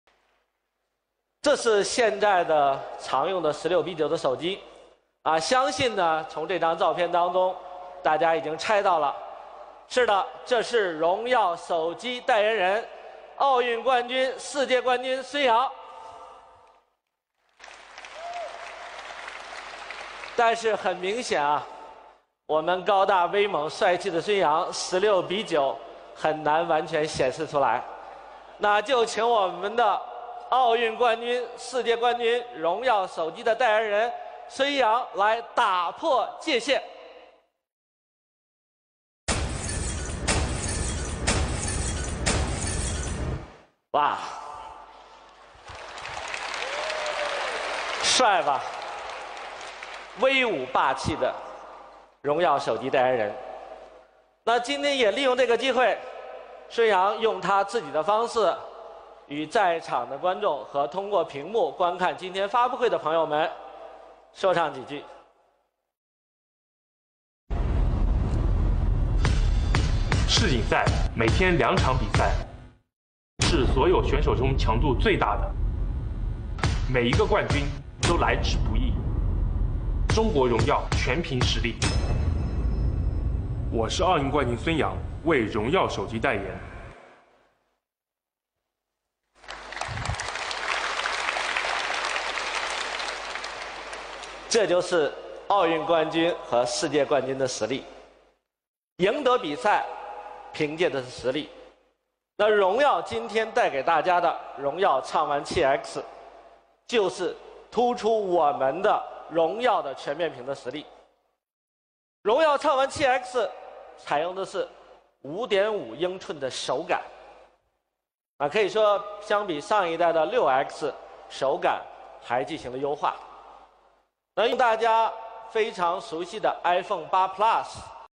【孫楊】【sunyang】【孙杨】荣耀畅玩7X手机发布會 代言人—孙杨
孙杨牵手荣耀 每一次都能引领各自领域
2017年初孙杨就与荣耀手机有过深度合作，当时孙杨是荣耀V9的速度特使，而今合作再度升级孙杨成为荣耀手机代言人。事实上，孙杨与荣耀手机的契合度非常高，作为世界超一流游泳运动员，孙杨以强大的实力和不断进取的精神征服了一个又一个对手，而荣耀手机则是以深厚的技术积累和不断的创新一次又一次的领先行业。